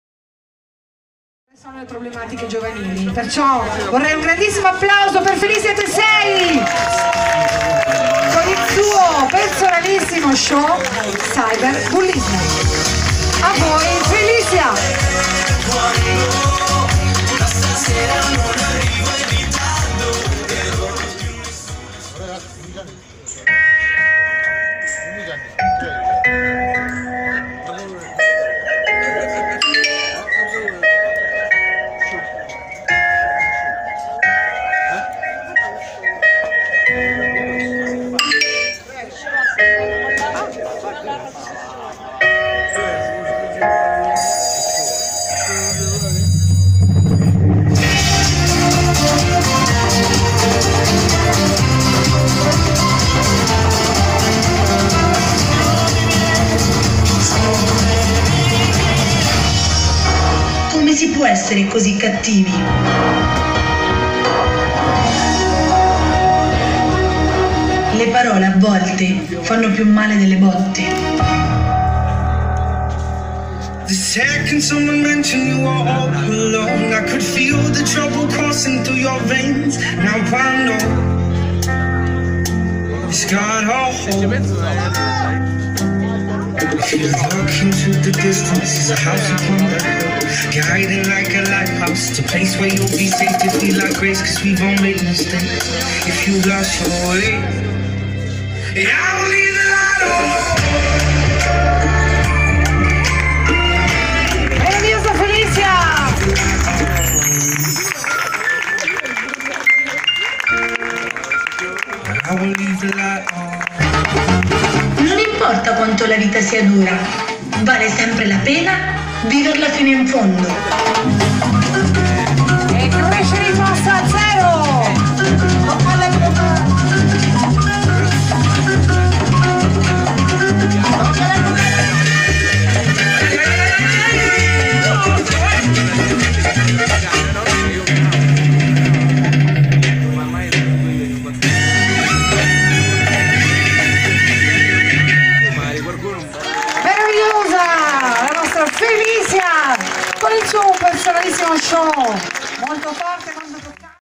domenica 18 agosto 2019 - caribbean show dance - Felicia Tesei
Special One Night - New Fashion Gia.Man.Dance -
Capolinea Chalet Ristorante New American Bar -
Lungomare Nord - Concessione 39 - Civitanova Marche - MACERATA